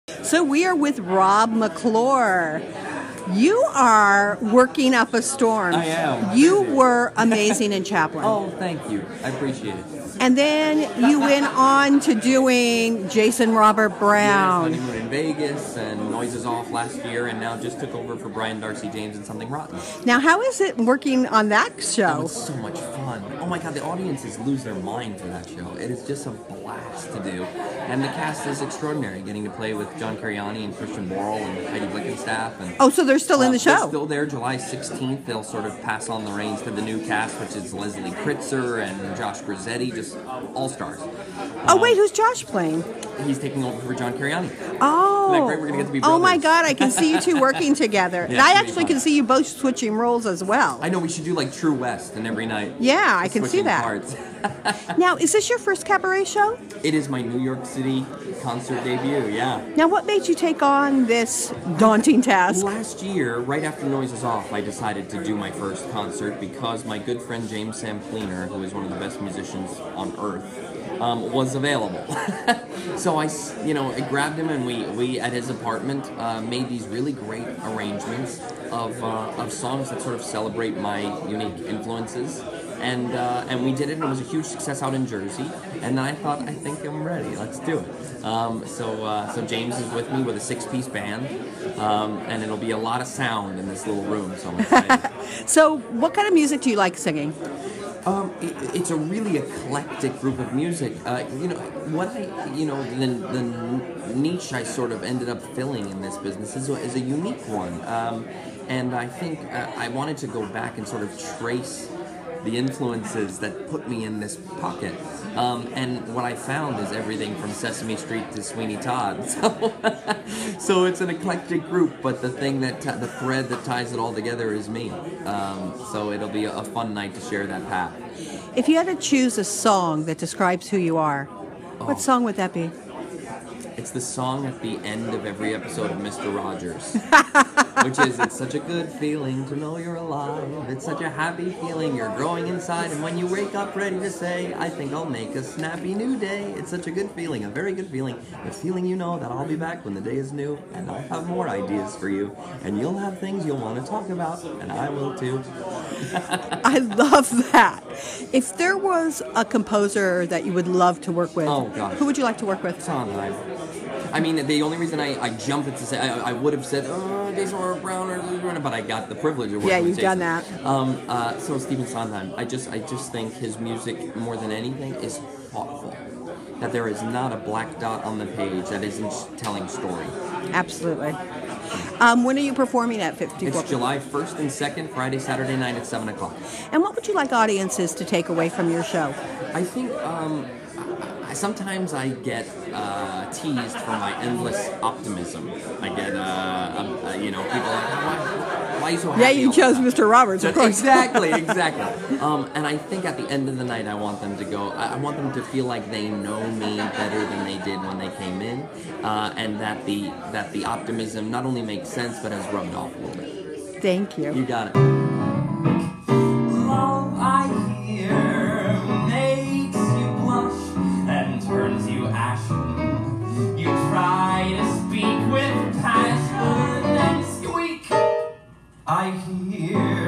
Rob McClure
T2C talks to Rob McClure days before he begins his New York City solo concert debut, acclaimed Tony nominee Rob McClure (Chaplin) celebrates his unique influences through story and song. Expect a swinging band, special guests (both people and puppets!) and lots of laughs and memories from his leading roles in Honeymoon in Vegas, Noises Off and Avenue Q! to his starring role in Something Rotten.

Featuring Special Guests:
Jenn Colella
Jennifer Barnhart
Rick Lyon
Matthew Scott

Musicians:
James Sampliner (Music Director,  Arrangements, Orchestrations, and Piano)
Alex Eckhardt (Bass)
Dan Berkery (Drums)
Jami Dauber (Trumpet)
Clint Sharman (Trombone)
Ben Kono (Reeds)

Rob McClure: Feinstein's/ 54 Below July 1 and 2 at 7pm